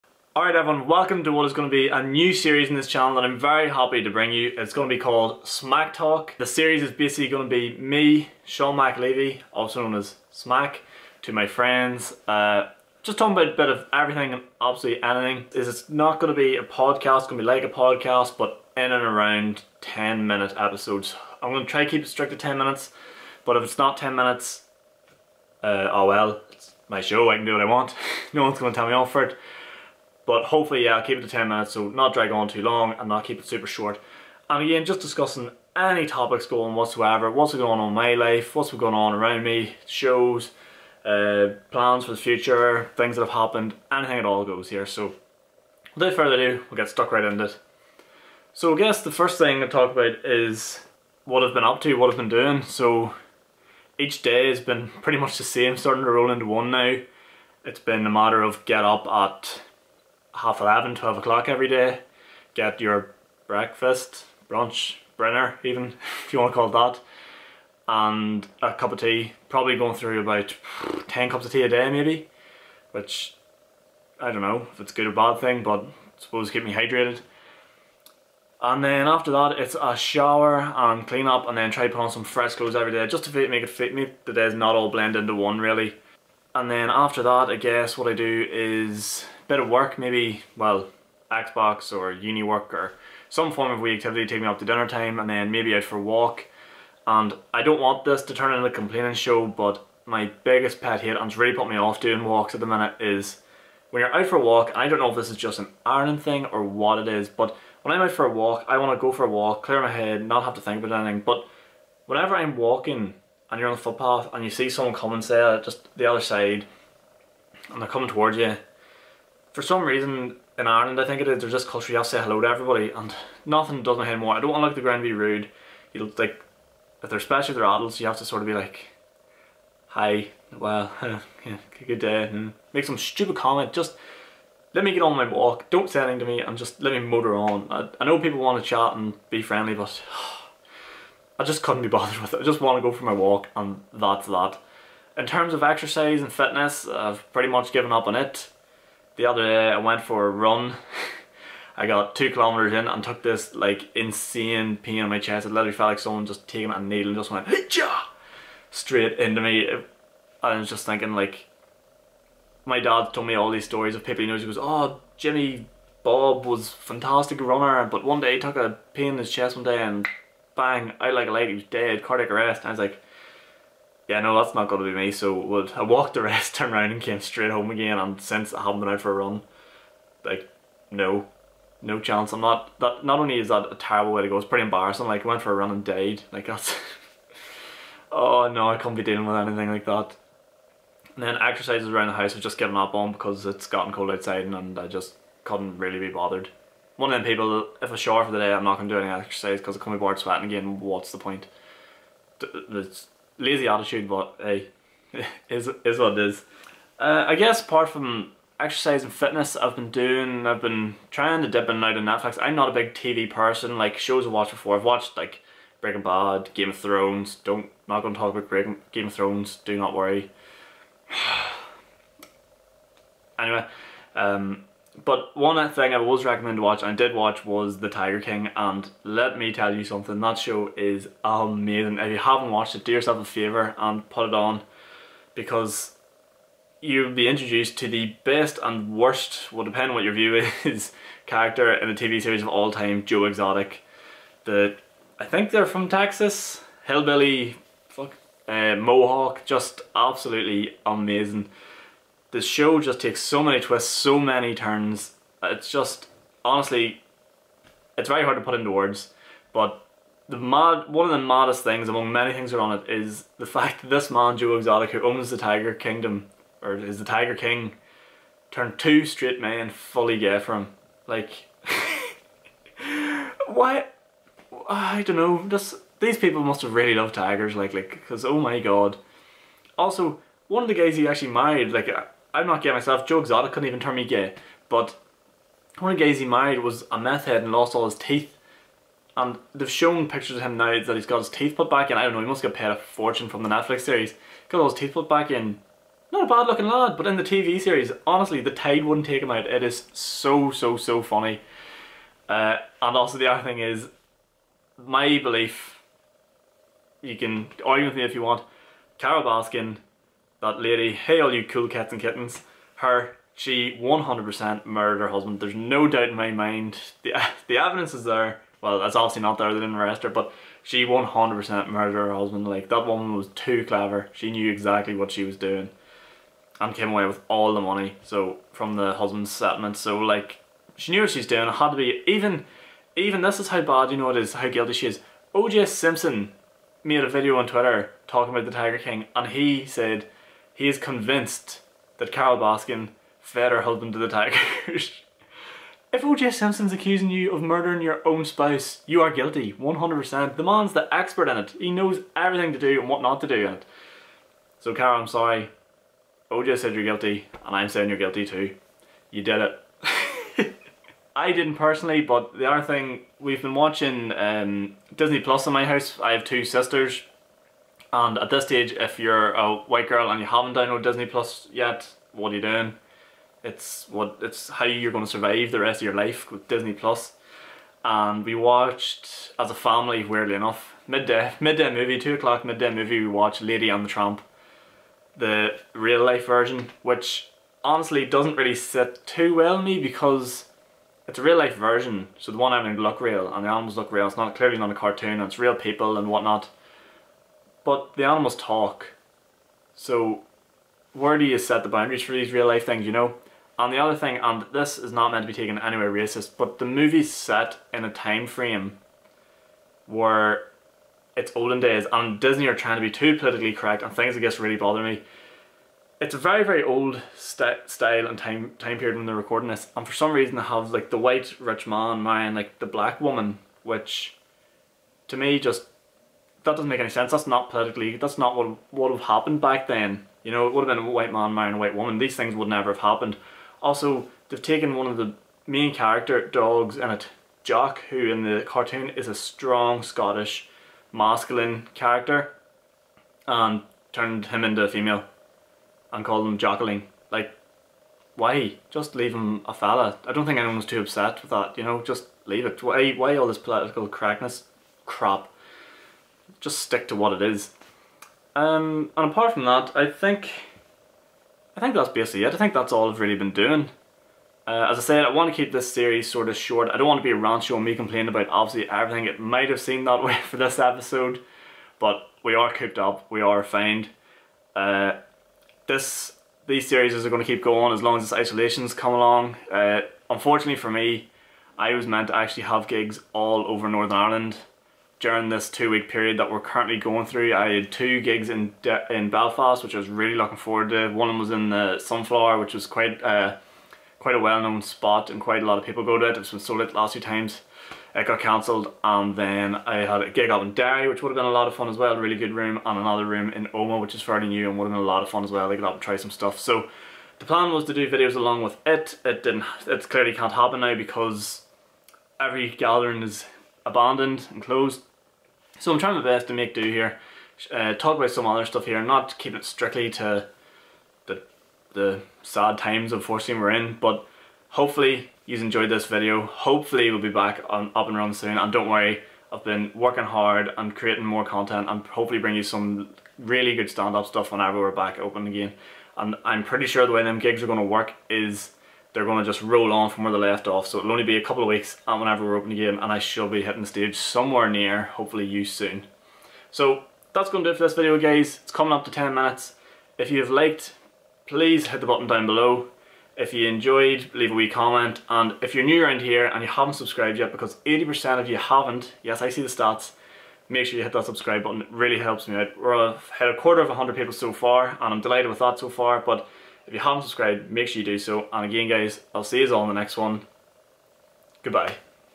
SMAC Talk - Episode #1 (Introduction, Tiger king, Disney+ & Why I Hate Isolation walks!)
SMAC Talk - Episode #1

My new series where I sit and discuss everything and anything within 10 mins! Topics from my life and the world around me. This week I discuss The Tiger King, Disney+, Isolation walks and much more!